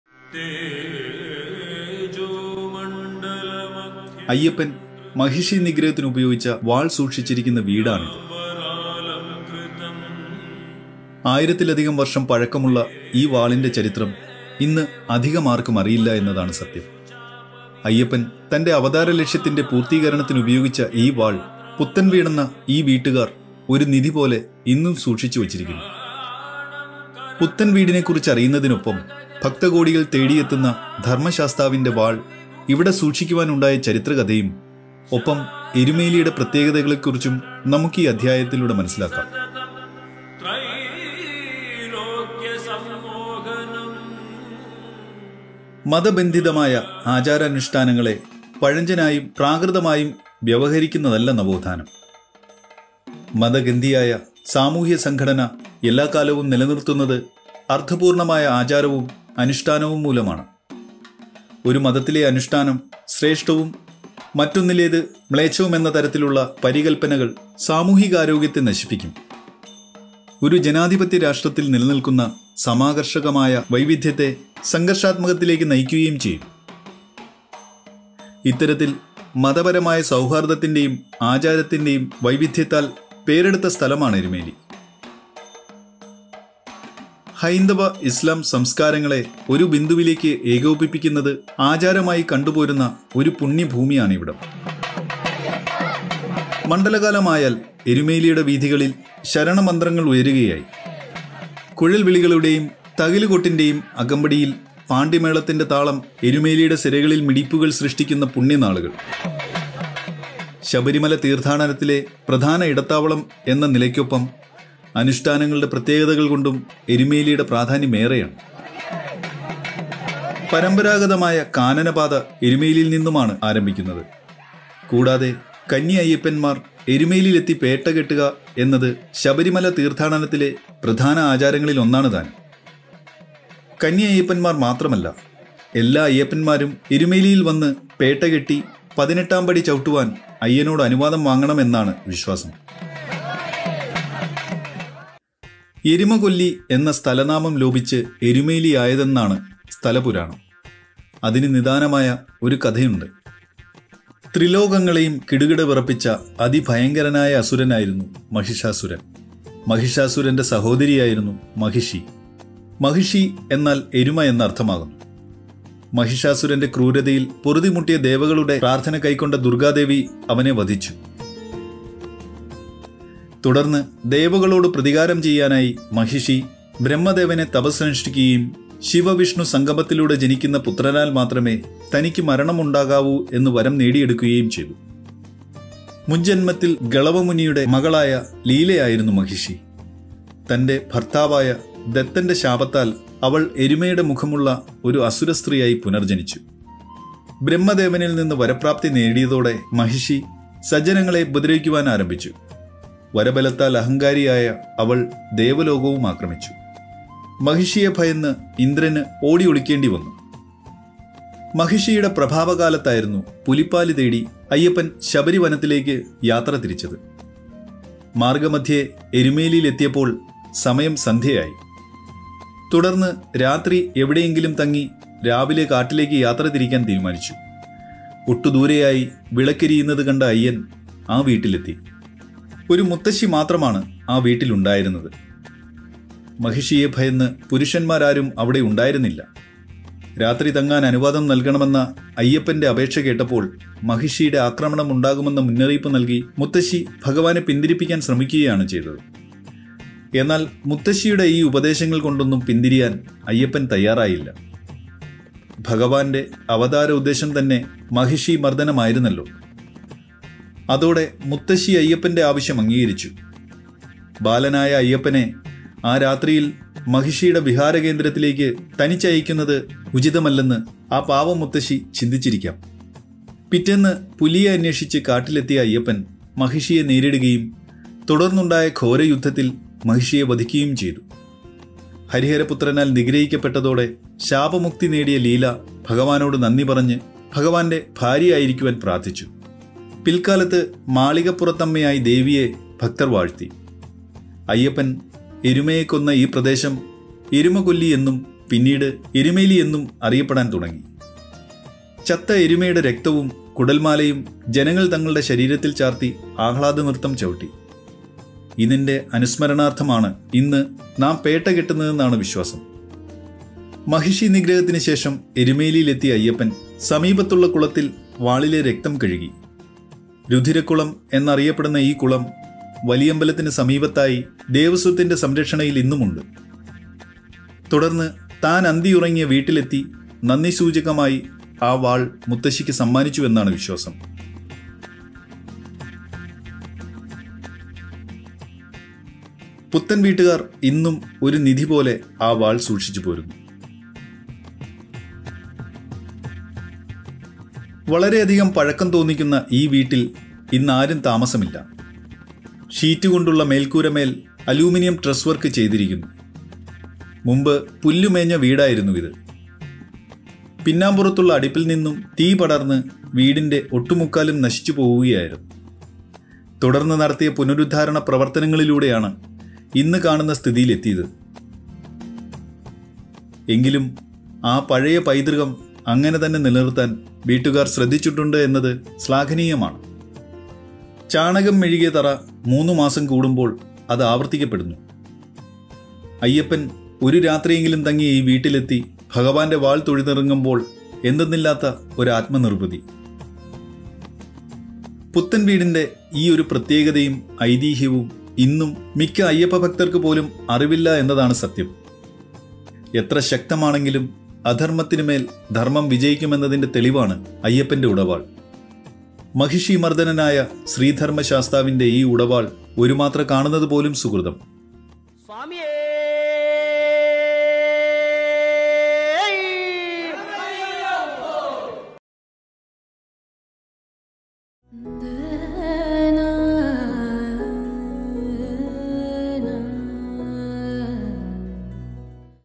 ശബരിമലയുടെ കഥ | Part 1 | Sabarimala Pilgrimage | Devotional Stories
Along with knowing the legendary story behind the Erumeli Petta Thullal, we will also see Ayyappan's sword in the video.

എരുമേലി പേട്ട തുള്ളലിന് പിന്നിലെ ഐതിഹ്യ കഥ അറിയുന്നതിനൊപ്പം അയ്യപ്പന്റെ വാളും നമുക്കീ വീഡിയോയിലൂടെ കാണാം.